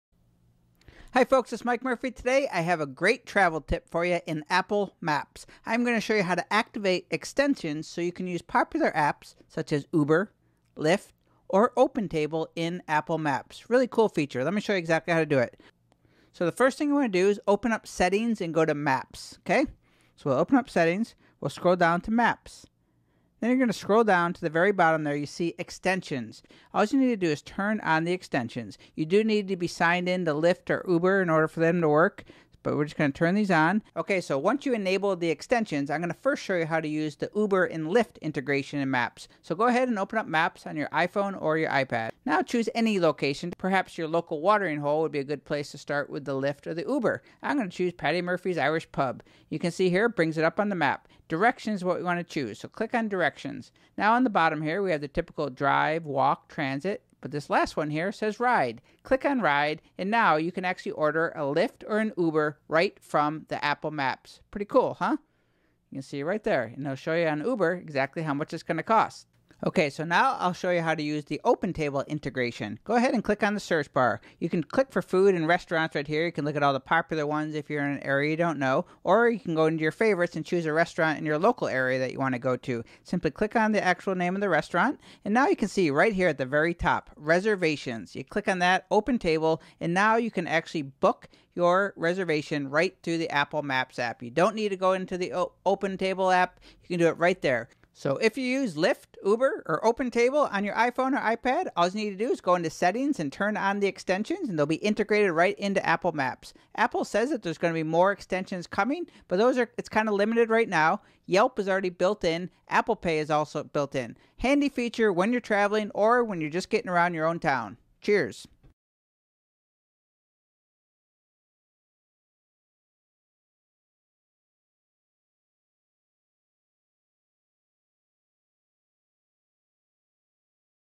How To Use Extensions (Uber, Lyft, Open Table) in Apple Maps
Quick Tip: Extensions in Apple Maps

Extensions are limited, but more coming

Uber, Lyft & Open Table Integration with Maps

Download Free Apps (Uber, Lyft and/or Open Table)

Sign in to Uber & Lyft

Maps
Scroll Down To Extensions
Turn on Each Extension

Open Maps
Click Search Bar
Select Food (just an example)
Click Popular
Tap on Restaurant

For Uber & Lyft:

Tap Directions
On The Bottom (Drive, Walk, Transit, Ride)

Select Ride
Book Lyft
Book Uber (price listed, 😎)

For Open Table:
Go To Restaurant
Reservations/Open Table right up Top
Click
Book Reservations right in Maps